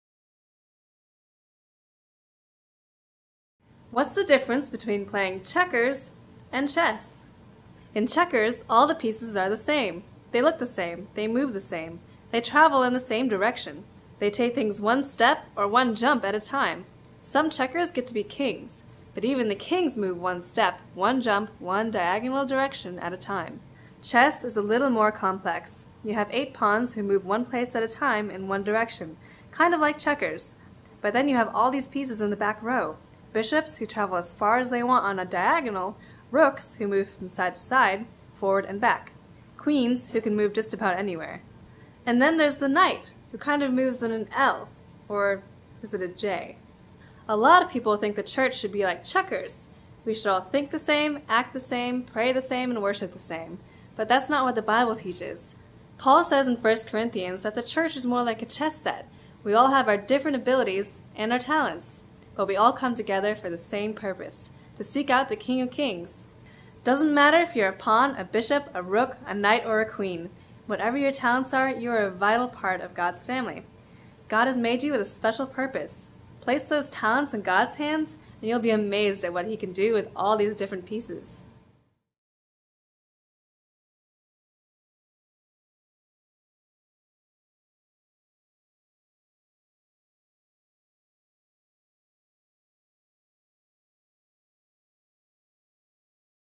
Object Lesson on Gifts and Talents - Chess & Checkers
Use chess and checkers to teach kids an object lesson on gifts and talents. Perfect to use during a Children's sermon, Sunday School, or Children's Church.

Children's Ministry Deals is the #2 Children's Ministry curriculum producer in the US.

❏ HELPFUL BLOG POSTS: ❏

❏ HELPFUL BLOG POSTS: ❏